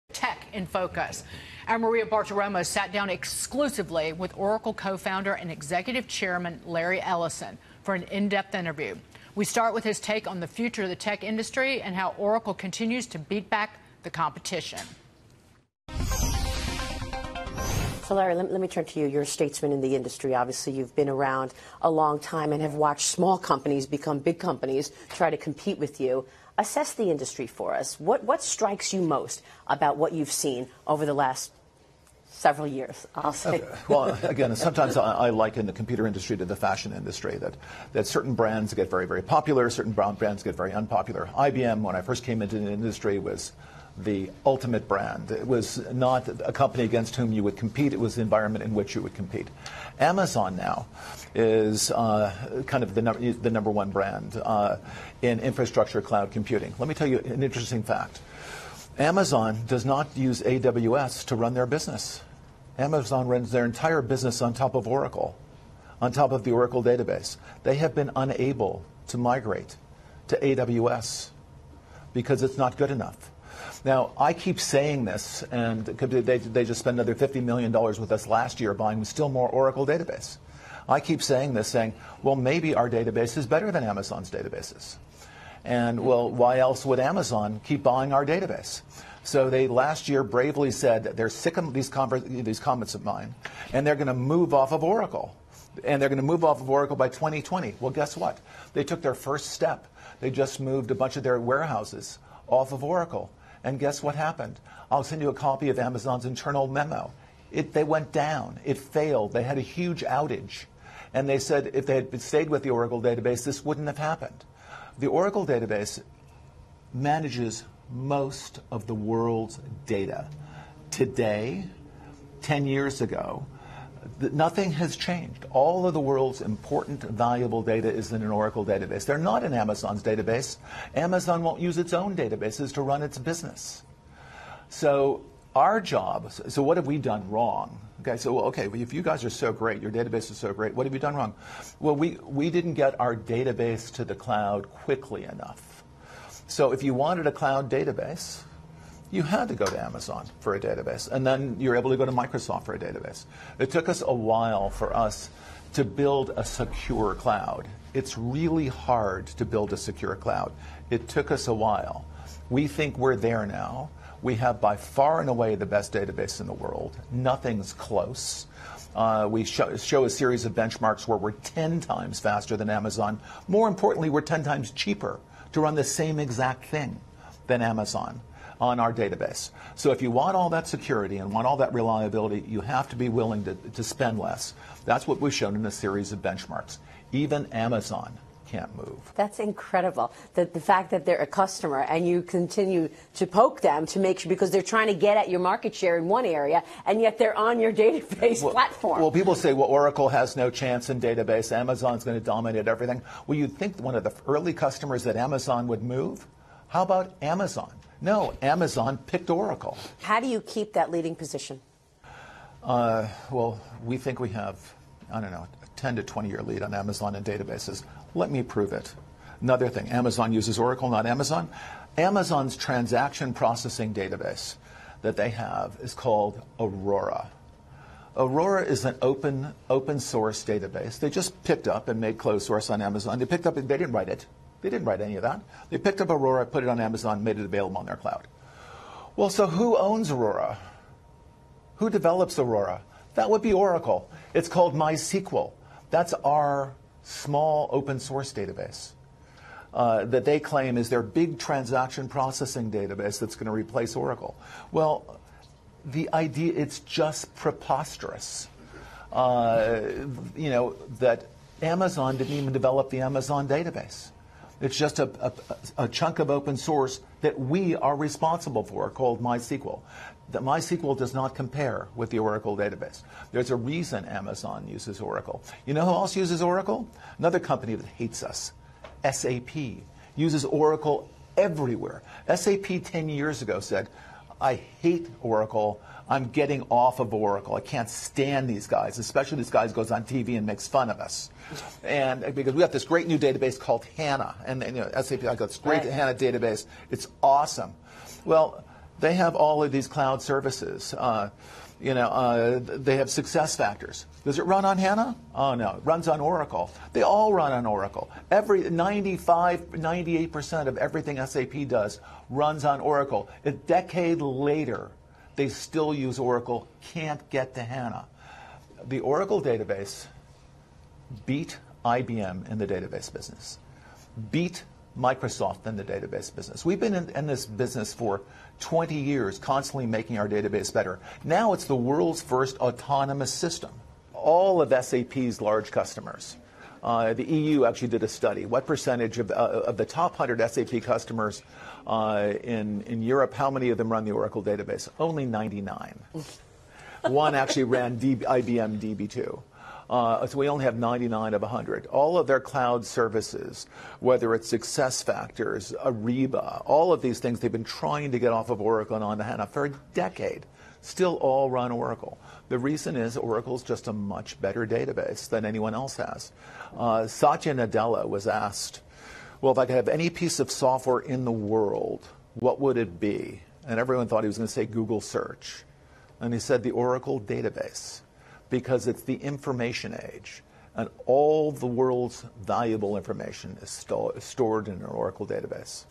Oracle database, not Amazon has most of the world's data: Larry Ellison
Oracle co-founder Larry Ellison on the success of the company's database versus competitors such as Amazon.